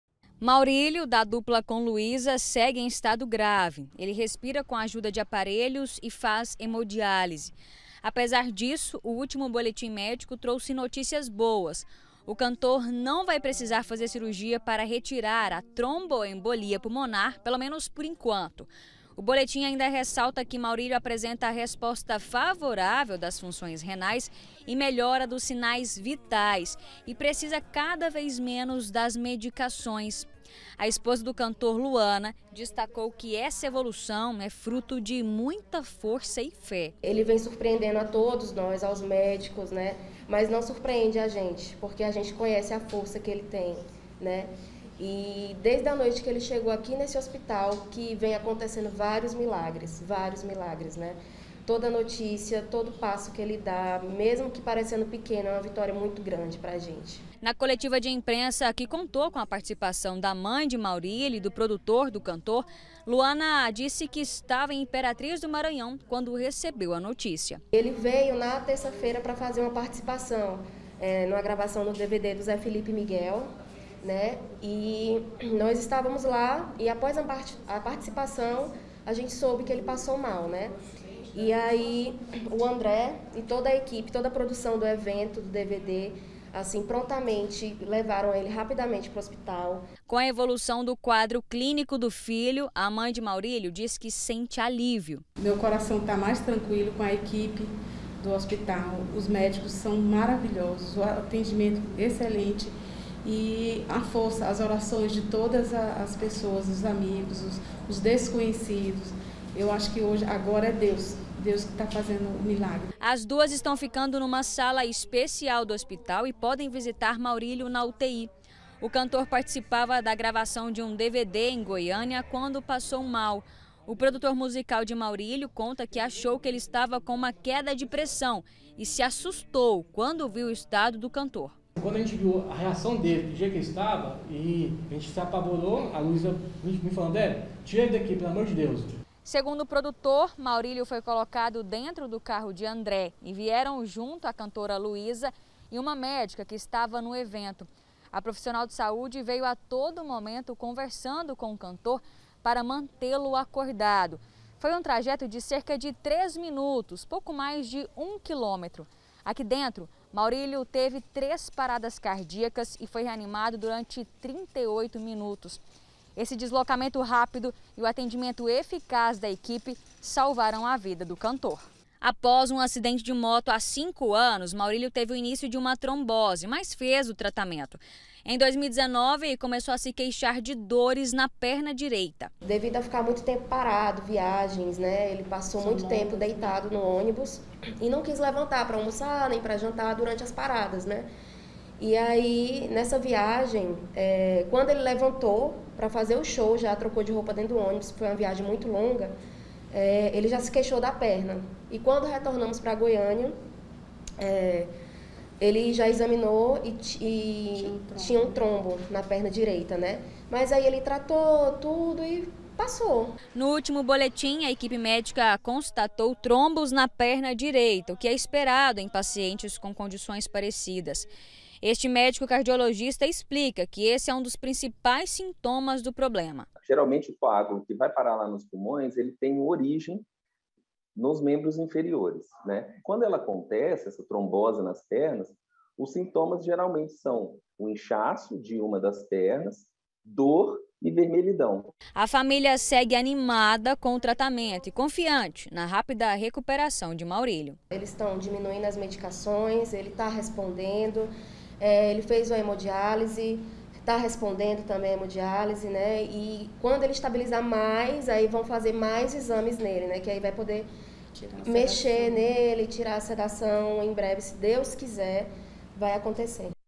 MAURÍLIO ESTÁ MELHOR: SEDAÇÃO É RETIRADA E CANTOR REAGE AO TRATAMENTO
O GOIÁS RECORD CHEGA AGORA COM AS ATUALIZAÇÕES DO ESTADO DE SAÚDE DO CANTOR SERTANEJO MAURÍLIO.